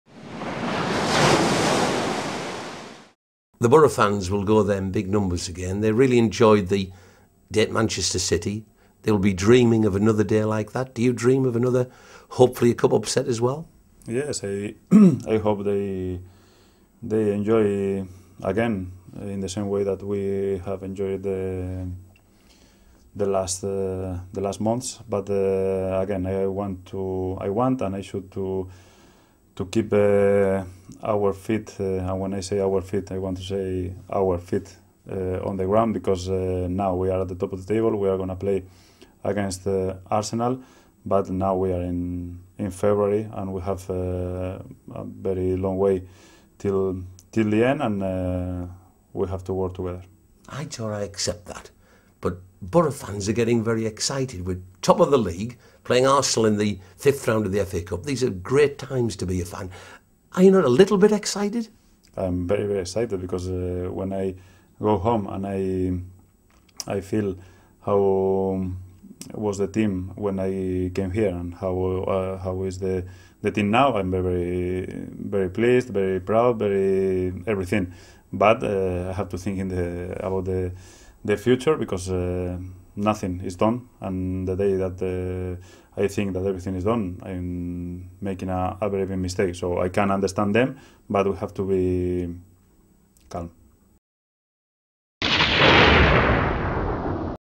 Arsenal v Boro: The Excitement Mounts!
This afternoon Boro take on Arsenal in the 5th round of the FA Cup and quite a few people are getting rather excited...